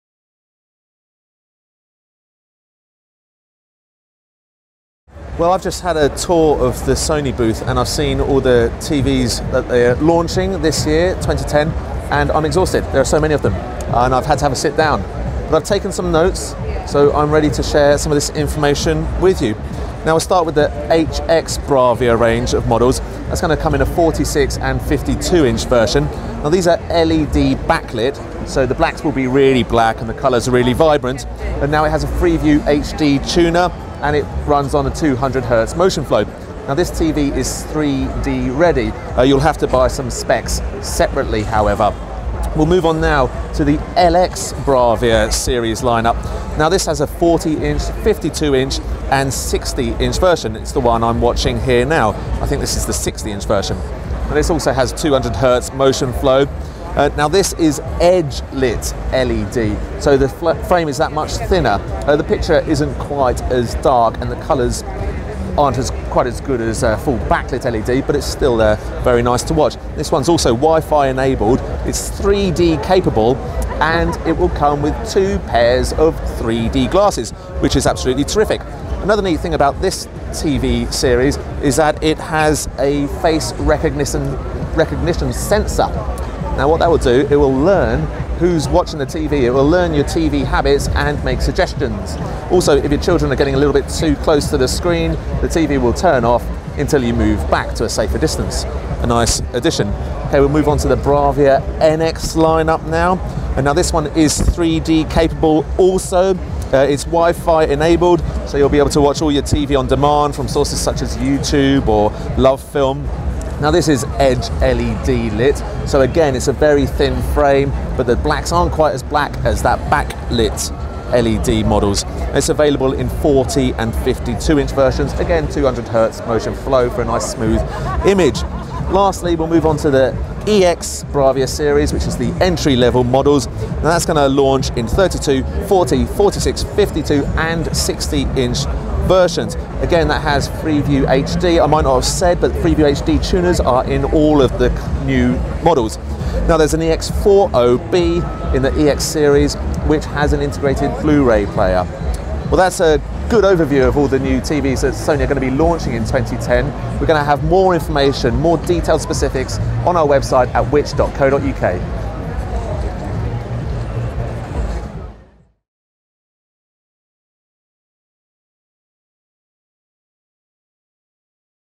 Which?: Sony NX, EX, HX and LX series 3D Ready TVs at CES 2010
We got a tour of Sony's 2010 NX, EX, HX and LX series range of TVs including many that are 3D ready.